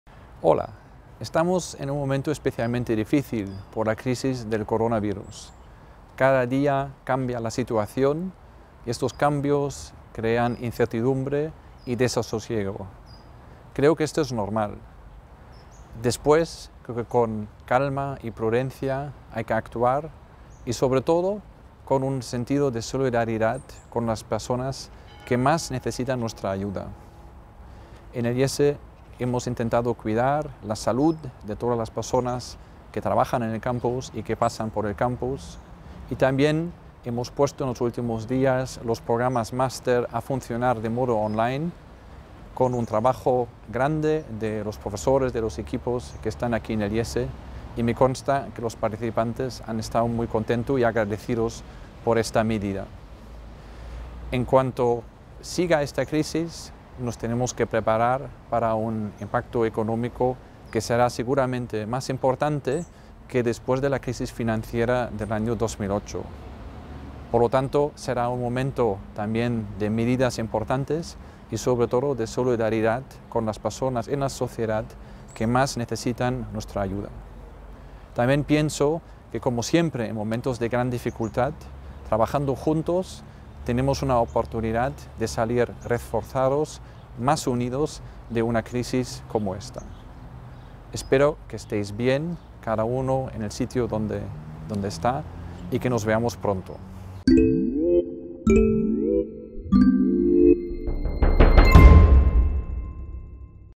Franz Heukamp: el reto del coronavirus
Nuestro Dean Franz Heukamp ha publicado un vídeo reflexionando sobre la crisis del coronavirus y cómo puede ayudarnos a ser más solidarios con todos.